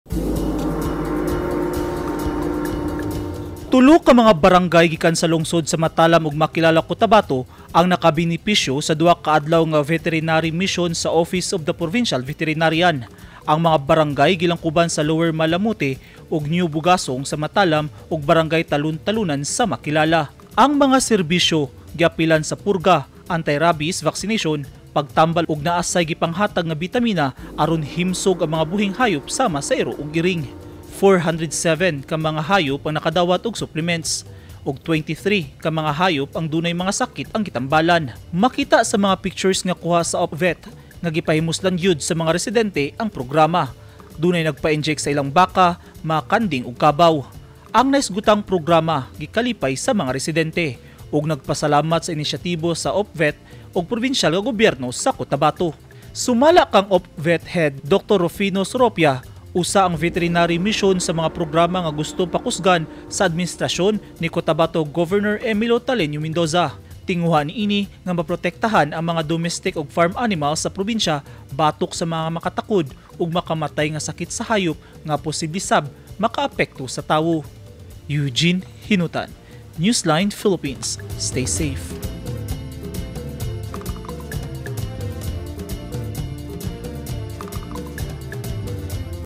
Kapin libo ka mga hayop sa Cotabato Province nakabenepisyo sa Veterinary mission
Gihatdan og serbisyo pinaagi sa veterinary mission sa office of the provincial veterinarian sa Cotabato Province ang tulo ka mga barangay sa lungsod sa Matalam ug Makilala. Apil sa mga serbisyo ang anti-rabies vaccination,purga ug paghatag og bitamina sa mga hayop